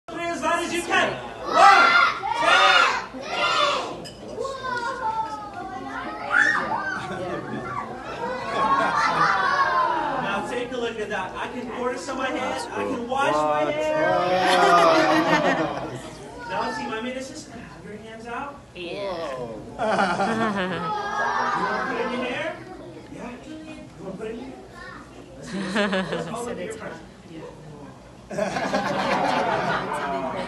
CRAZY PURIM PARTY IDEA (2023)
A dry ice show is a fantastic way to bring science and celebration together during Purim festivities for kids. By using dry ice to create fog and other eye-catching effects, this show can easily pique children's interest in the properties of matter and the principles of science. Additionally, Purim is a holiday that encourages joy and happiness, and a dry ice show can heighten the excitement and fun of the occasion. As a result, this type of show can leave a lasting impression on kids and provide them with a unique and engaging way to learn about science while enjoying the Purim holiday.